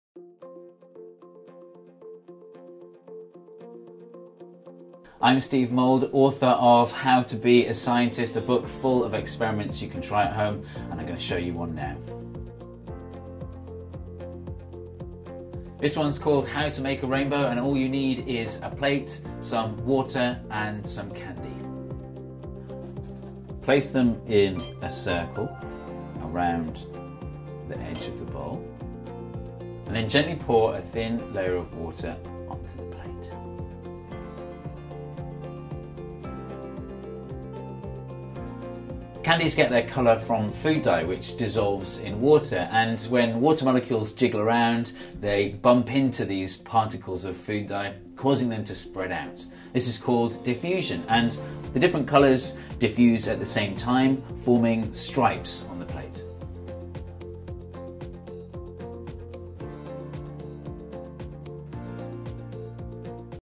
How To Be a Scientist US: Make a Rainbow
Steve Mould, author of How to Be a Scientist, brings you an awesome science experiment for kids that you can do at home.

Using everyday household objects including a plate, candy, and water, you can make a colorful rainbow appear!

Plus Steve Mould will explain the science behind the experiment. Being a scientist isn't just about wearing a lab coat and performing science experiments in test tubes. It's about looking at the world and trying to figure out how it works.